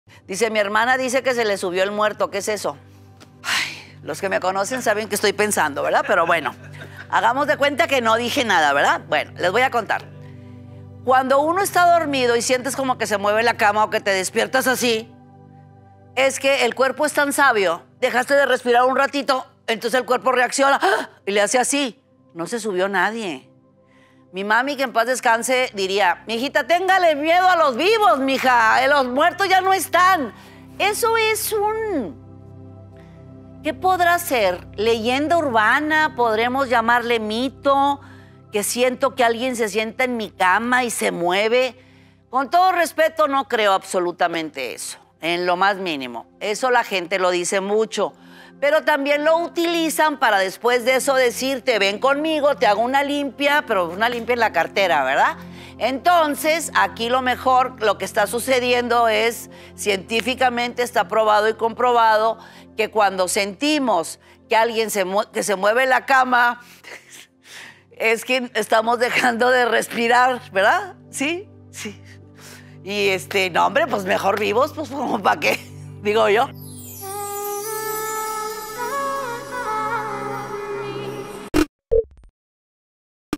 ¡Se Me Subió el Muerto! Tips y Consejos de Mizada Mohamed.
🌟Me escribieron preguntando que quiere decir cuándo dicen "Se me subió el muerto" Todo tiene una razón de ser, aquí te lo cuento.